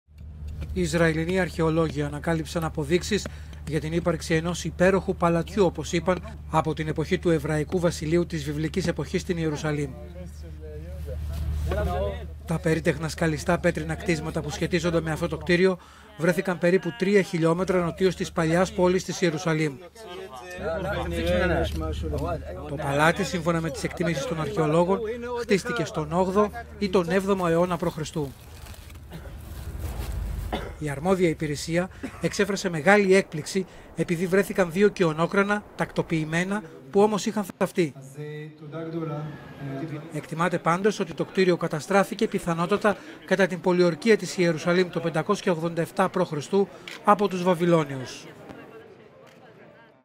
Αρχαιολόγοι ανακάλυψαν ευρήματα ενός «υπέροχου παλατιού» νοτίως της Ιερουσαλήμ
Εκτιμάται πως πρόκειται για ένα παλάτι του 8ου ή του 7ου αιώνα π.Χ.